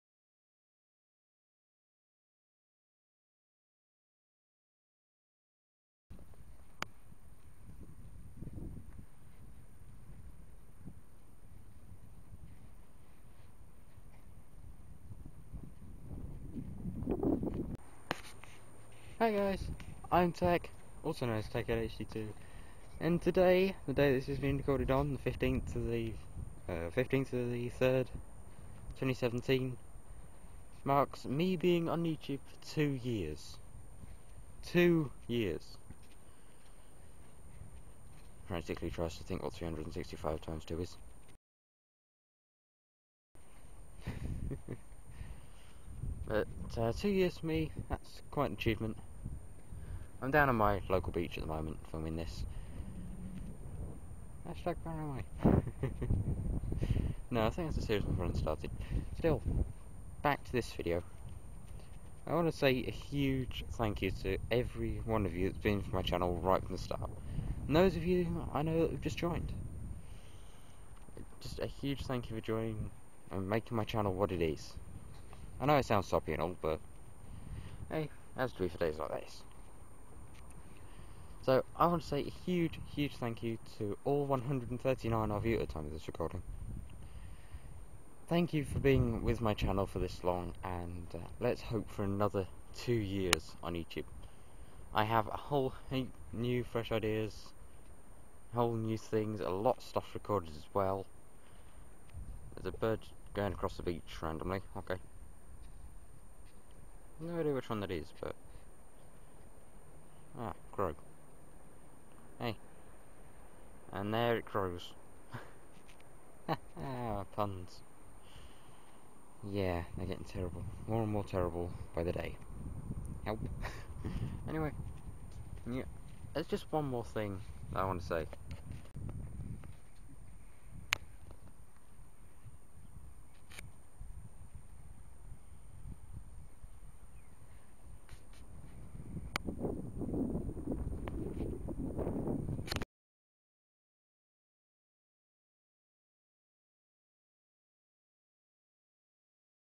THANK YOU! | 2 Years On Youtube!! [2YRSOFTECH][Re-Uploaded/Edited]
2 years on yt? I have to say a huge huge thanks to each and everyone of you for all your support over my time here.
HUGE Thanks go out to
Mum's Corner(for helping me get to where I need to be)
TheProGamerGT(for being in my videos and letting me help his)
and..
LEGACY 2-yes thats how its spelt-For offering to make music for my channel and being a mate.
To this very day I remember my first ever sub-Sneffel from Italy.
Thank you once again to everyone and heres to two more years!